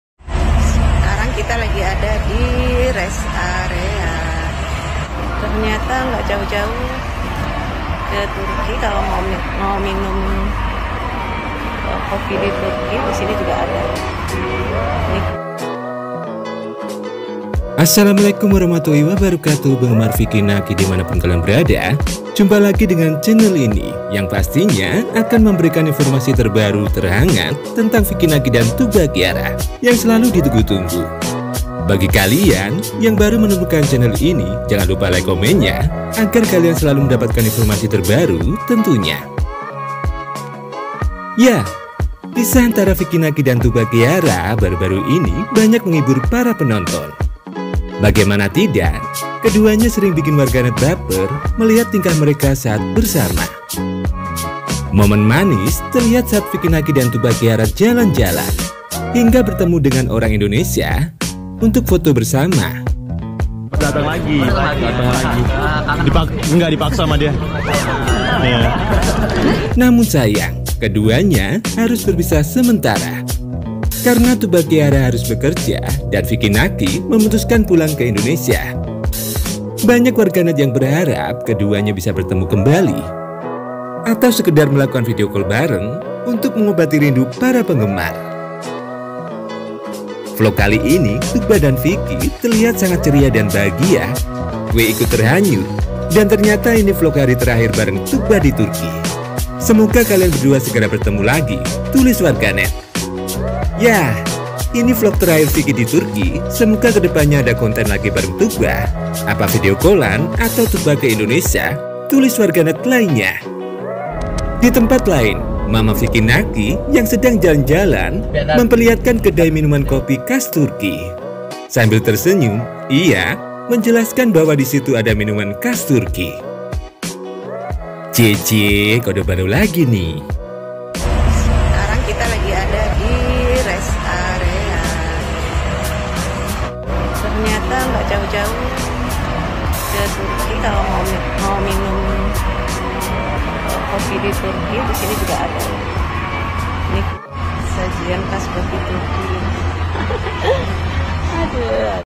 SEMUA KANGEN LIHAT FIKI NAKI DAN TUGBA KIARA MAMA FIKI NAKI BERI KODE INI | TUGBA KIARA HARI INI
fiki naki hari ini 3 September 2022

SEMUA KANGEN LIHAT FIKI NAKI DAN TUGBA KIARA MAMA FIKI NAKI BERI KODE INI